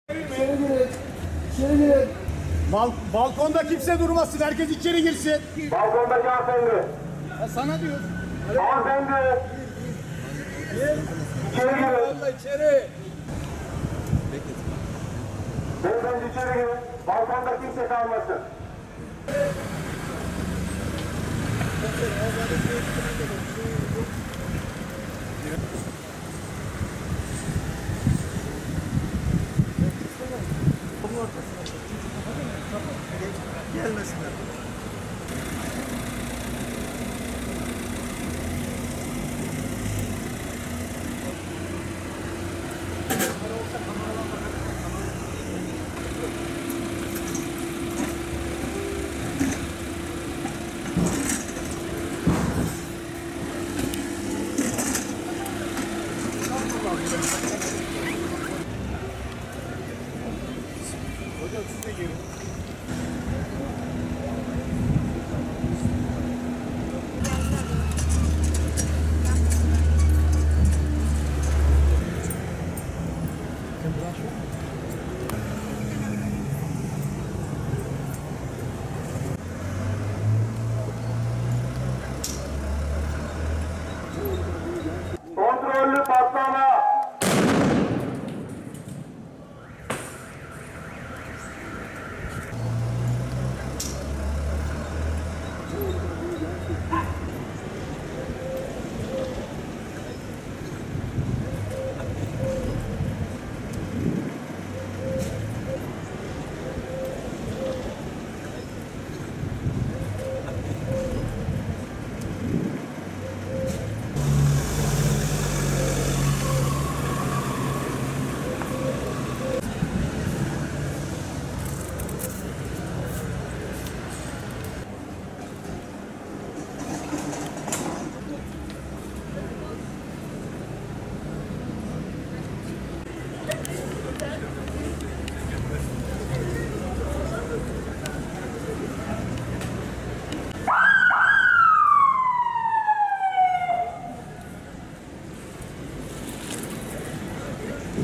Balkondaki kadına polisten ‘şüpheli valiz' uyarısı
Aydın'ın Efeler ilçesinde bir işyerinin önüne bırakılan şüpheli valiz ekipleri harekete geçirirken, çevre güvenliği alan ekipleri en çok meraklı vatandaşlar zorladı. Olayı izlemek isteyen ve balkonlara çıkan vatandaşların can güvenliği için sık sık içeri girmeleri konusunda anons yapan ekipler, bir kadının içeriye girmesi için uzun süre uğraş verdi. Ekiplerin ‘şüpheli valiz' uyarılarını dikkate almayan kadın, balkondan olanları izlemeye çalışsa da ekiplerin uzun uğraşları sonucu içeriye girdi.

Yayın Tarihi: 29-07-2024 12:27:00